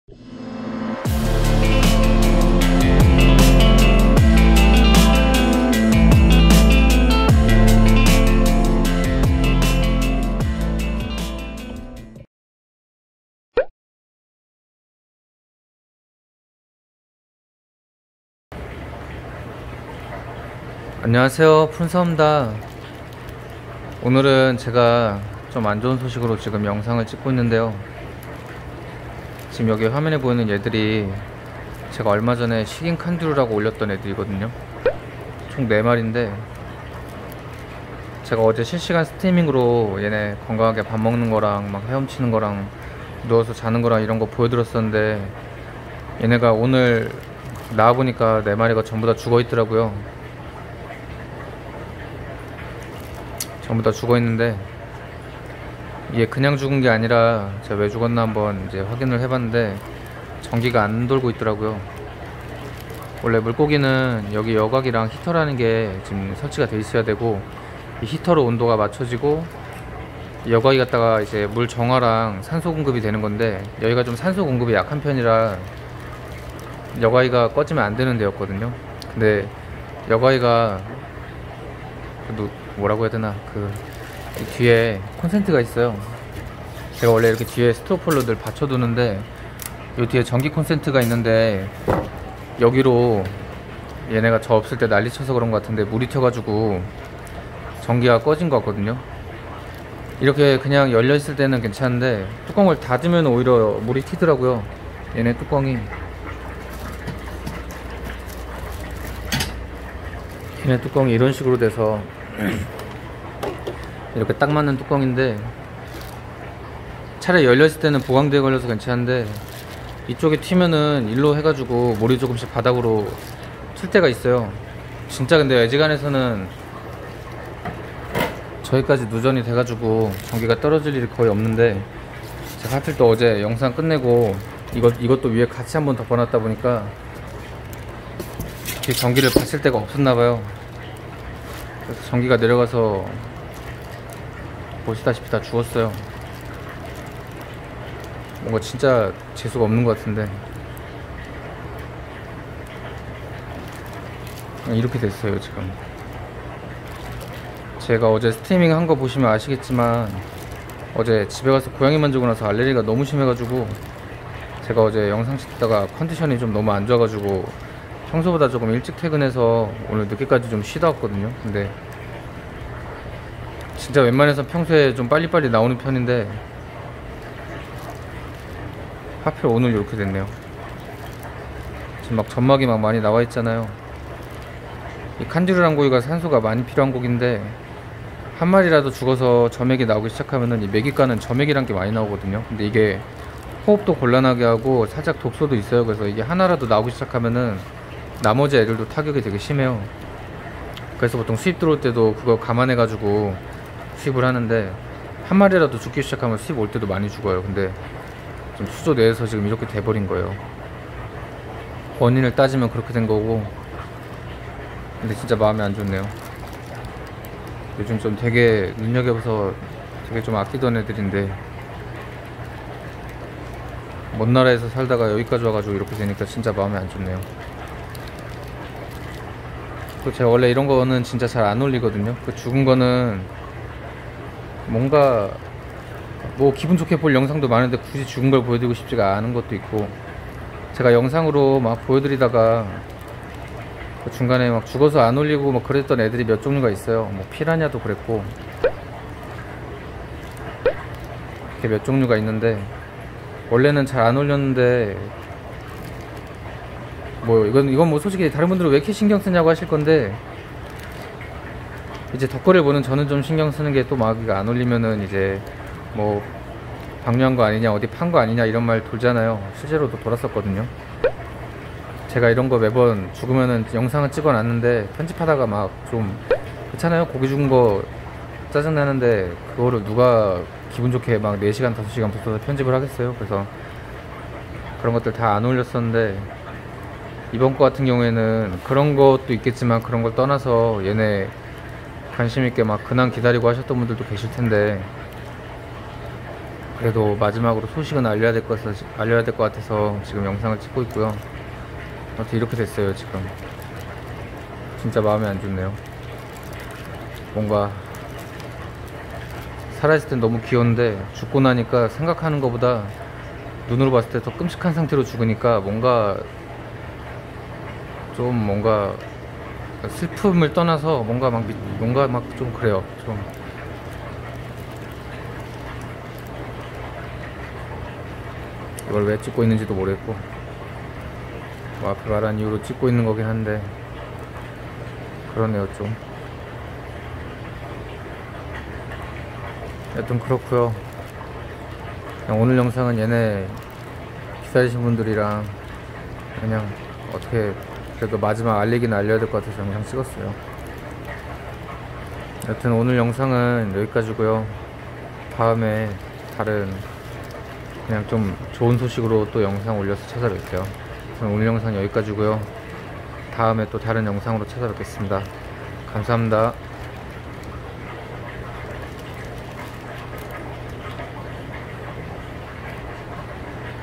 식인칸디루가 하늘나라로 떠났습니다...
식인칸디루가 5월31일, 모두 하늘나라로 떠났습니다.
너무 허무하게 보낸 아이들이고, 죽을당시 너무 끔찍한 상태라 올려야할지 말지 정말 고민을 많이했습니다.

고기에게 미안하기도 하고, 편집내내 저도 스트레스를 너무 받아서, 며칠간 영상에서 손을 놓기도 했었고.
편집자분께 부탁드릴까도 했지만, 이번영상은 제가 편집하는게 맞을것 같아서, 정말 꾹 참고 편집했습니다...

원래 아이들 밥먹는영상 올려드린다고 구독자분들께 약속드렸었는데.
이렇게 안좋은 소식을 올리게되서 다시한번 죄송합니다...